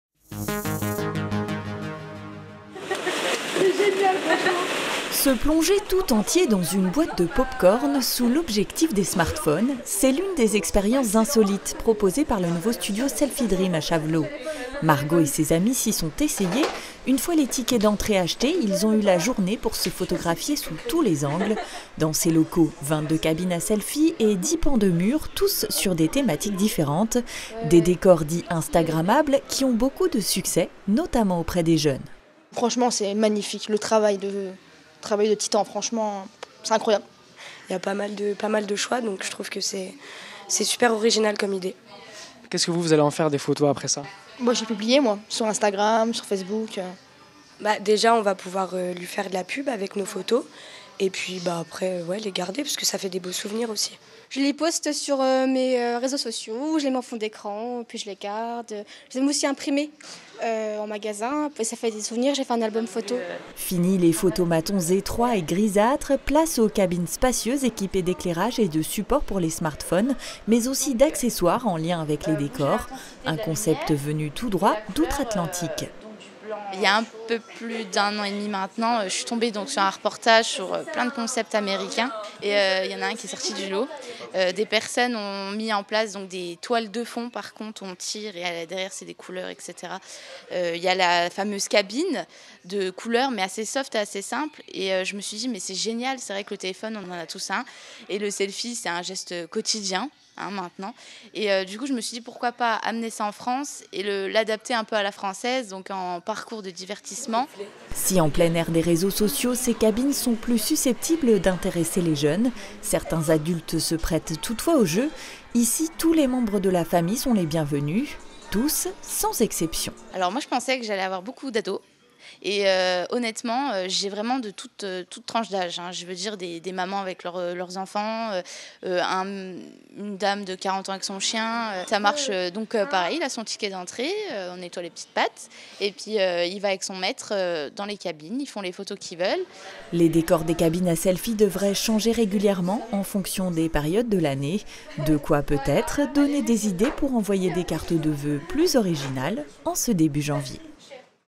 Paparazzi amateurs
Le selfie est à l’honneur à Chavelot. Une trentaine de décors, dont des cabines à selfies dans un studio de 450m2, sont à disposition des jeunes et moins jeunes.